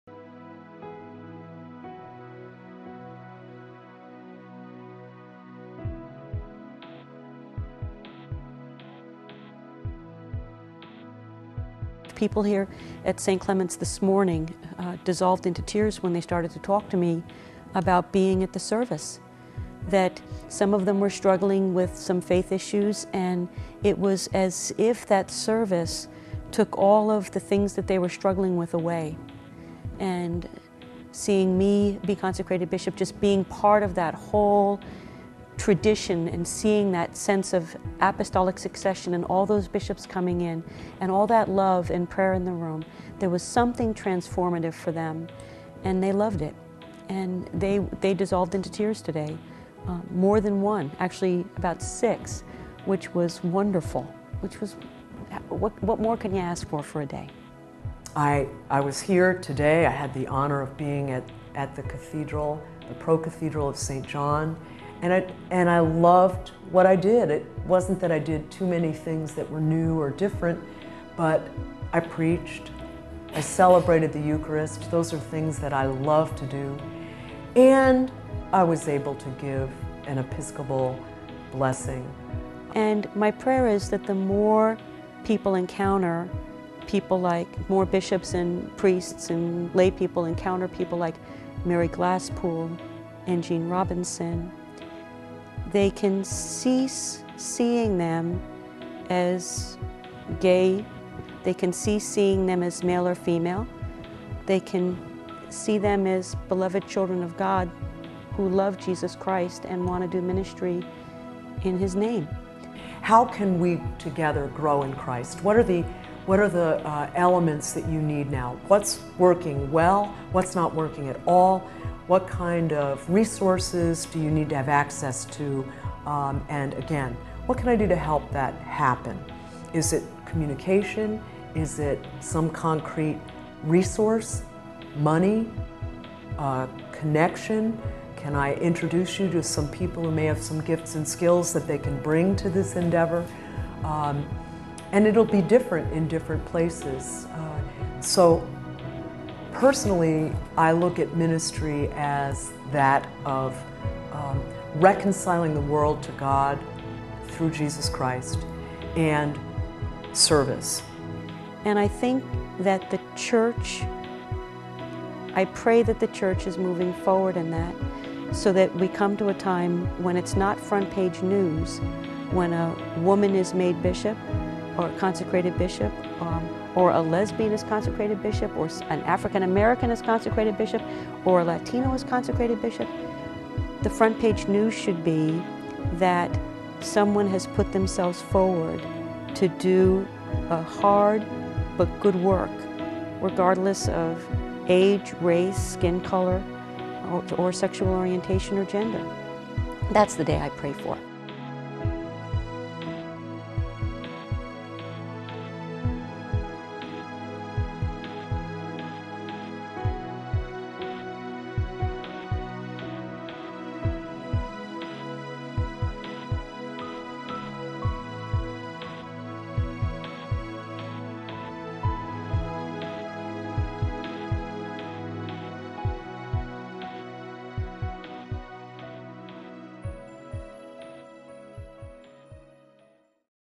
Bishops Suffragan
This week's Just Action video includes interviews with Bishop Suffragan Diane Bruce and Bishop Mary Glasspool as they began their new ministries on May 16 with visits to, respectively, St. Clement's Church, San Clemente, and the ProCathedral of St. John.